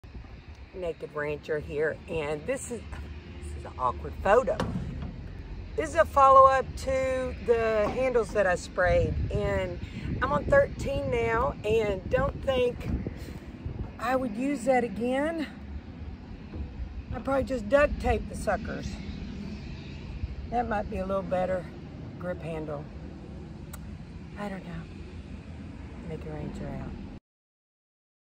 Plastidip follow up on wheelbarrow handles
Plastidip update on wheelbarrow handles. 13 coats, and still not exactly what I wanted. Guessin’ duct tape mighta been a better choice for this job.

It was a foggy morning and it looks like I cut my hair off…I haven’t. Looks can be deceiving.